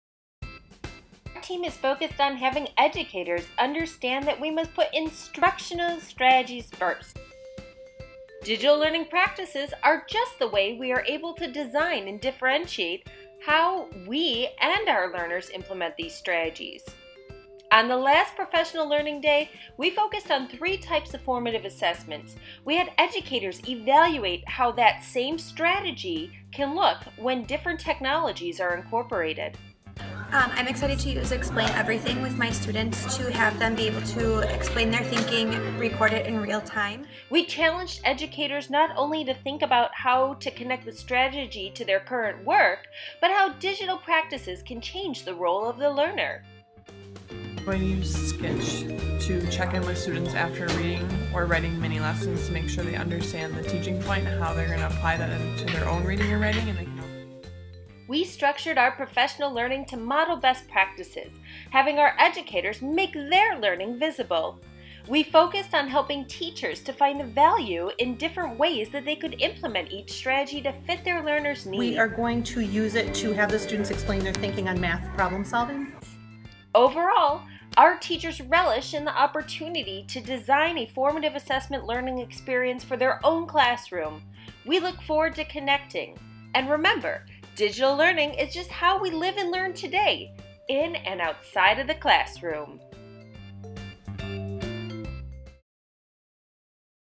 Formative Assessment Strategies Meet Digital Learning Practices
Our team is focused on having educators understand that we must put instructional strategies first.  Digital learning practices are just the ways we are able to design and differentiate how we and learners use implement these strategies.

On the last professional learning day we focused on 3 types of formative assessment. We had educators evaluate how that same strategy looks when different technologies are incorporated. We challenged educators to not only think about how to connect the strategy to their work, but how digital practices can change the role of the learner.

We structured our professional learning to model best practices, having our educators make their learning visible!  We focused on helping teachers to find value in the different ways they could implement each strategy to fit their learners’ needs.

Overall, our teachers relished the opportunity to design a formative assessment learning experience for their own classroom! We look forward to connecting!
Remember, Digital Learning is Just how we live and learn today- in and outside of the classroom!